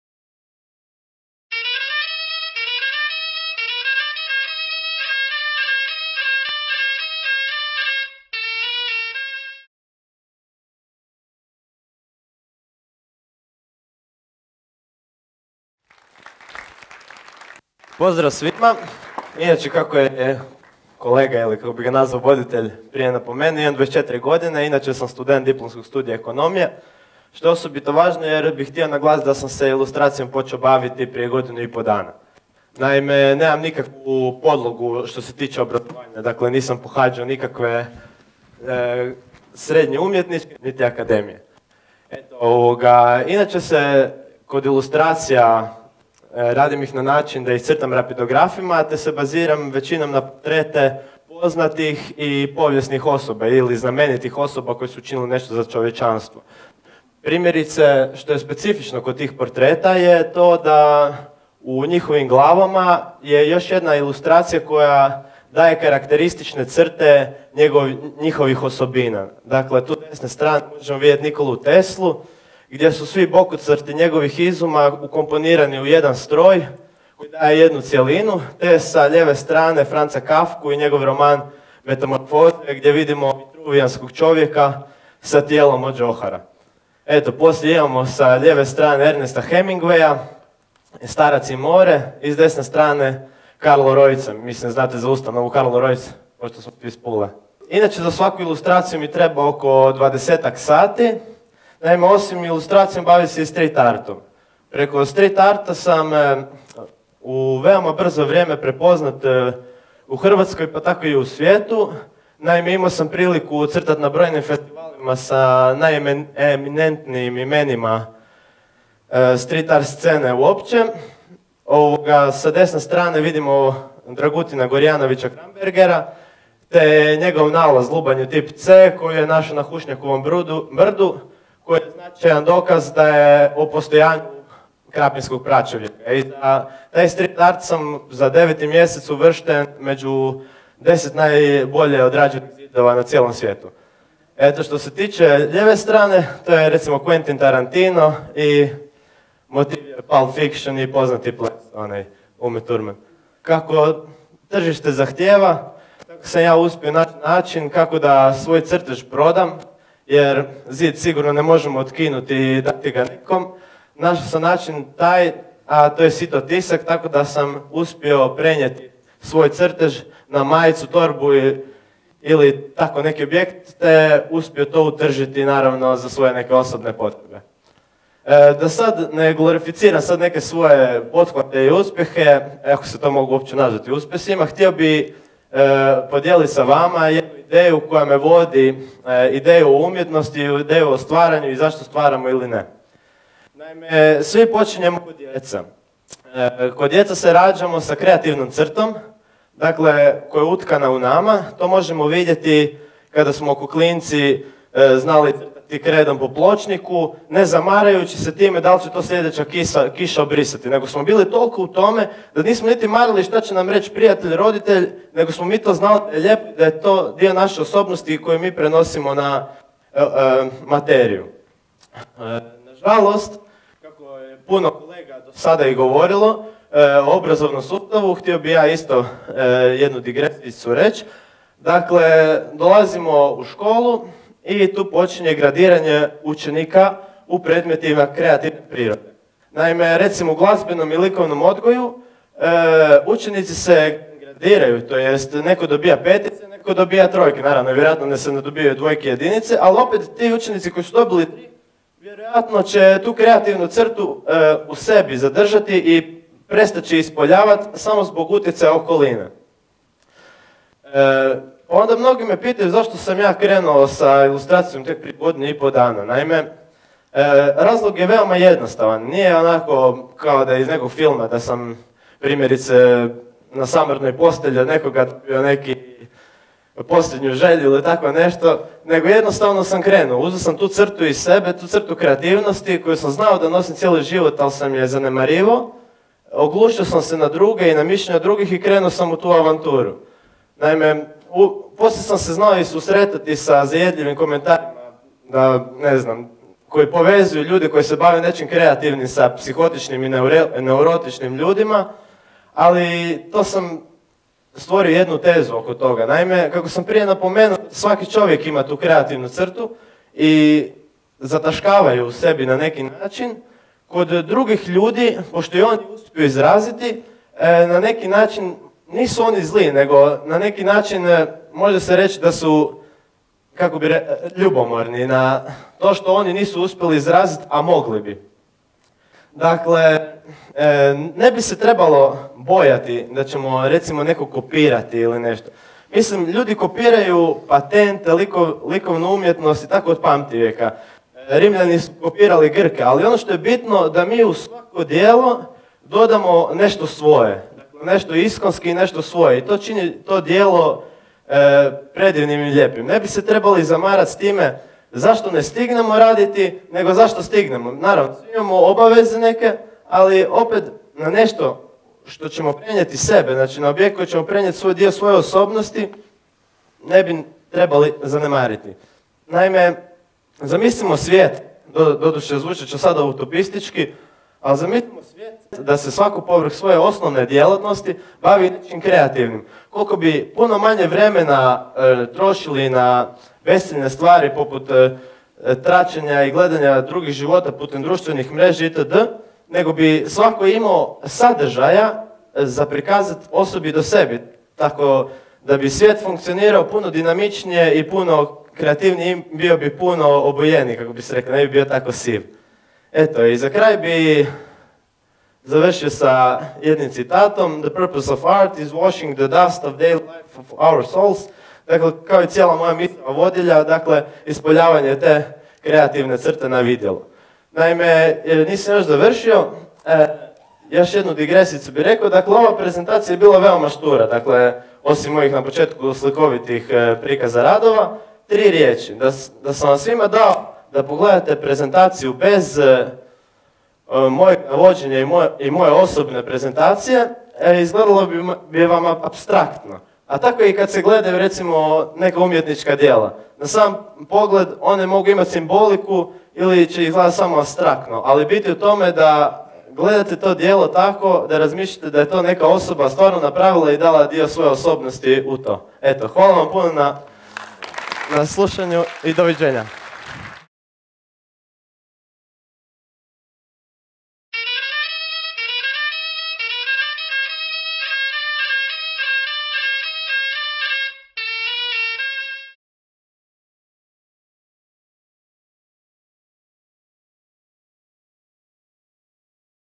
U pravokutniku | Vedran Štimac | TEDxPula
"Htio bi podijeliti sa vama jednu ideju koja me vodi, ideju o umjetnosti, ideju o stvaranju i zašto stvaramo ili ne.
Svi počinjemo kao djeca, kao djeca se rađamo sa kreativnom crtom koja je utkana u nama Kao klinci smo znali crtati kredom po pločniku ne zamarajući se time dal' će to sljedeća kiša obrisati nego smo bili toliko u tome da nismo marili što će nam reći prijatelj, roditelj, nego smo znali da je to dio naše osobnosti koju mi prenosimo na materiju."
TEDxPula - 17. listopad 2015.

Vedran Štimac ili poznatiji kao Polite BastART, 24 godišnji je samouki ilustrator iz Pule. Crtanjem se bavi od ranog djetinstva, no unazad par godina aktivnije. Svoje umjetničko stvaralaštvo temelji na ilustracijama te street-artu. Uglavnom se bazira na surrealistične ilustracije u kojima koristi dvije boje. Za ilustracije koristi rapidografe. Od streetarta radi murale, stikere, wheatpaste... Kod svake ilustracije bilo da je na zidu ili na papiru nastoji izgraditi priču, tj da ilustracija ima "glavu i rep". Ove godine crtao je na Boombarstick festivalu u Vodnjanu - na kojemu je imao i samostalnu izložbu, na Indirekt festivalu u Umagu, te je imao prilike izložiti svoje radove u Istarskoj sabornici u Poreču u sklopu Profuture. Od ostalih izložbi izlagao je na DeVilleovom usponu i na fort Turtianu